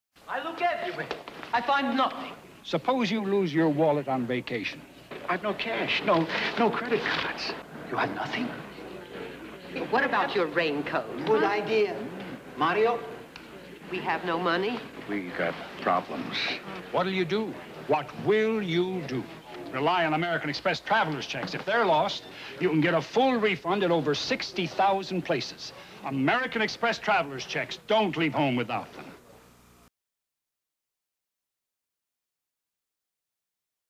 American Express 'Lost Wallet' Commercial (Karl Malden, 1978)
"Don't leave home without them."
American Express Travelers Cheques commercial featuring actor/spokesperson Karl Malden. Spot aired November 1978. for 1970s pop culture fun.*